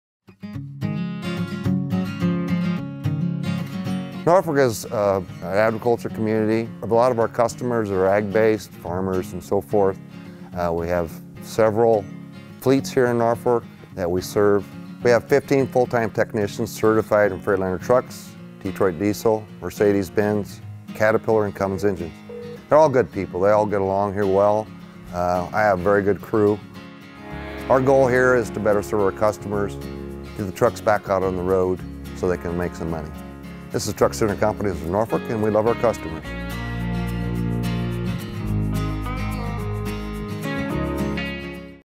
Truck Center Companies - Norfolk
Watch to hear Service Manager, Steve Coolidge explain how his crew takes care of customers at Truck Center Companies - Norfolk.